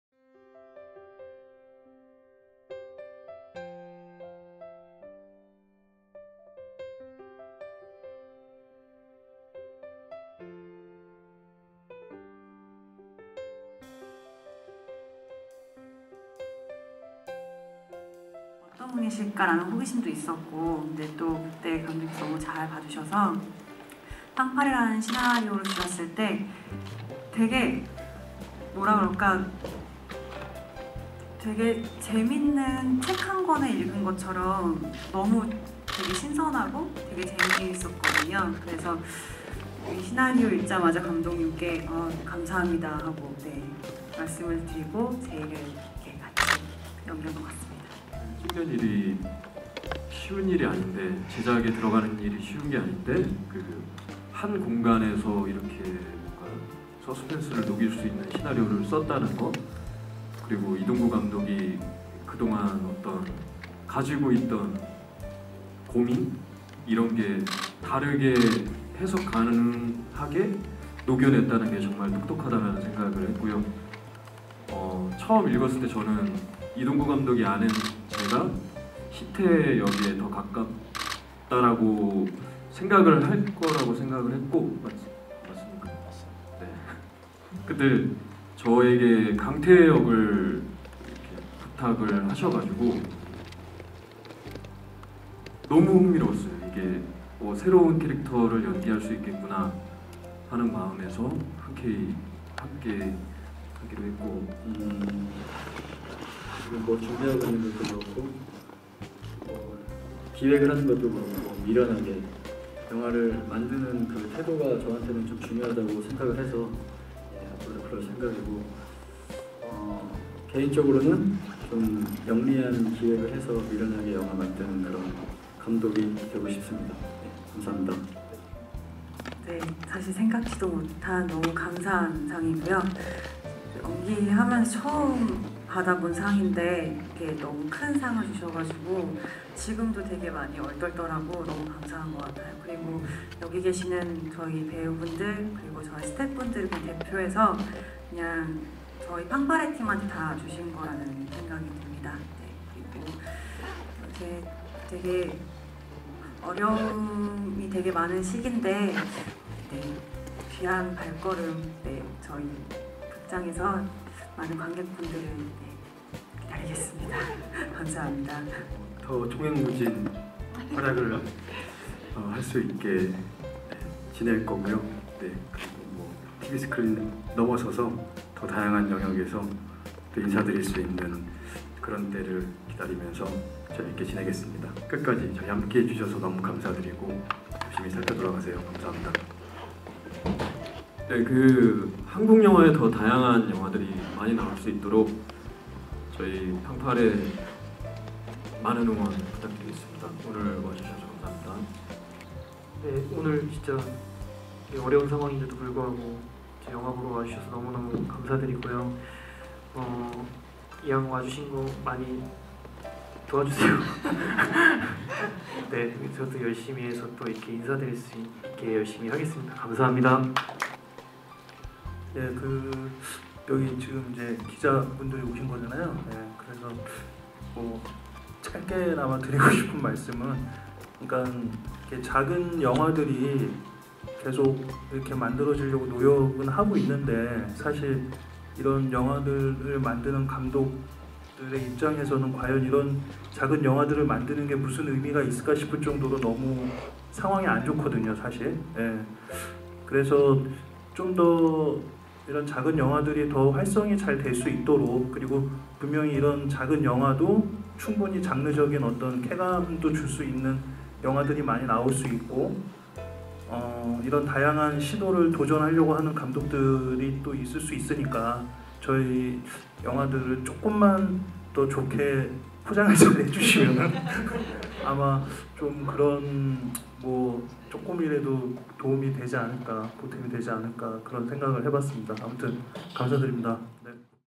영화 ‘팡파레’ 언론시사회 신선한 영화를 보고싶다면!
이돈구 감독과 임화영,박종환,남연우,박세준,이승원등의 배우들이 23일 오후 서울 용산CGV점에서 열린 영화 팡파레 언론시사회에 참석했다.